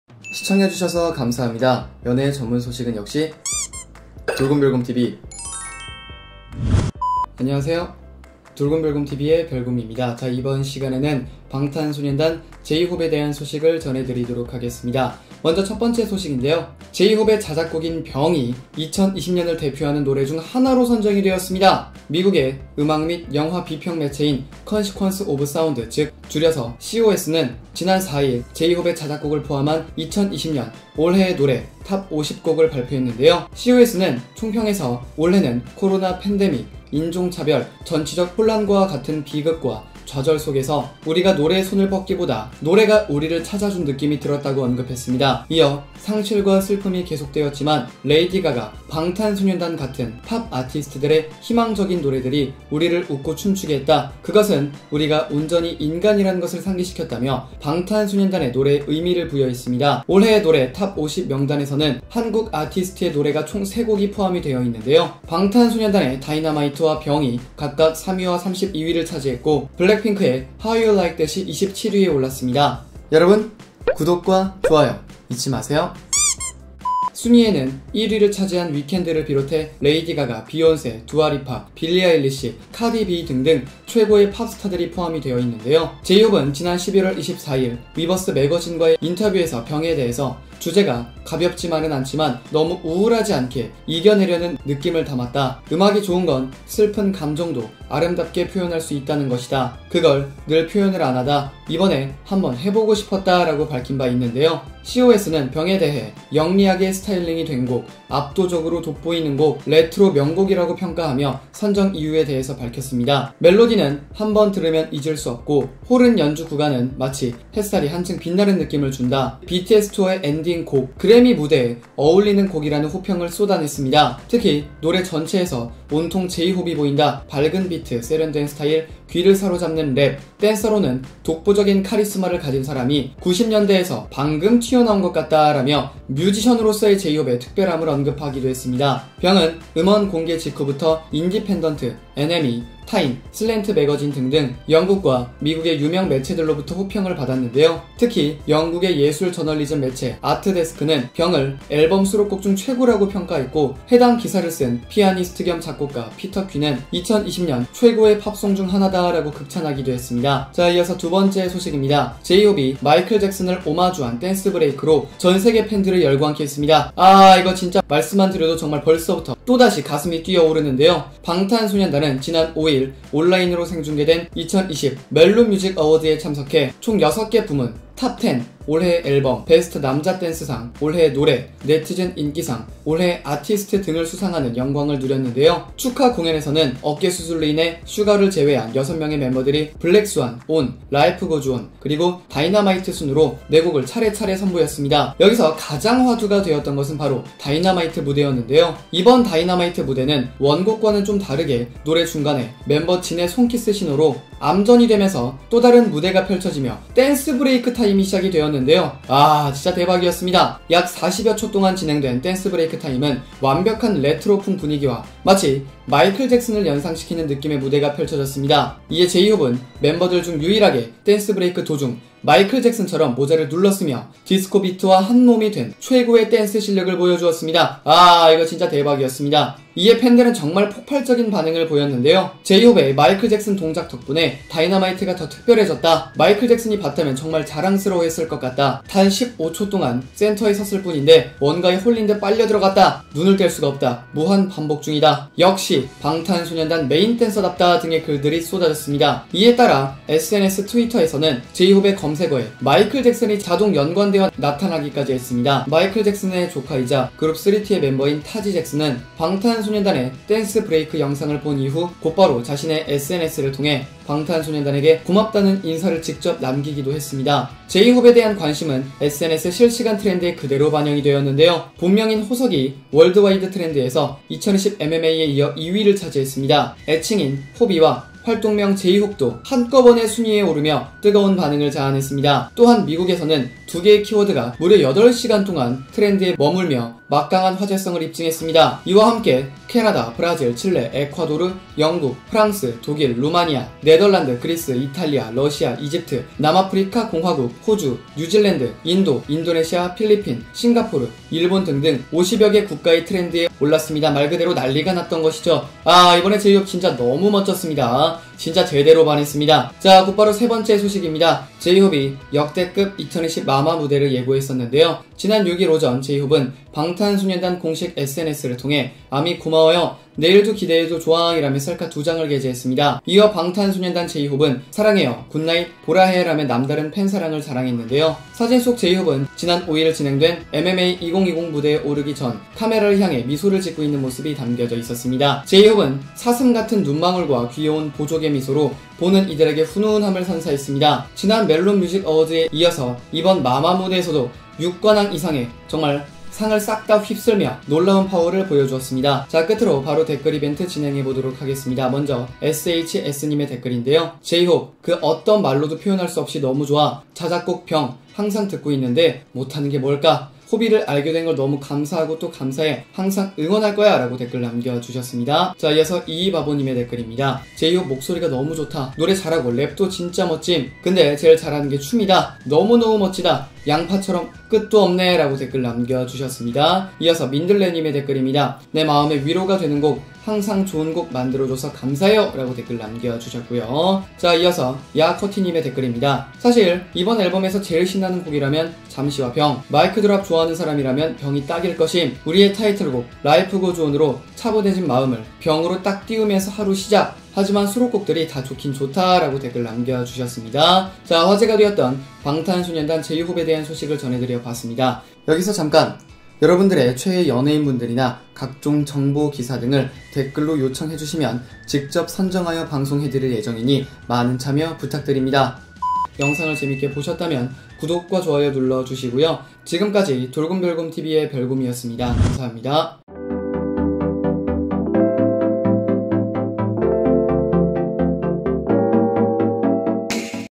【ENG】"美친 무대" BTS 제이홉, 마이클잭슨 완벽 댄스브레이크에 전세계 난리인 상황ㄷㄷ BTS j-hope 돌곰별곰TV
정대규

감사합니다.

감사합니다^____^

감사합니다.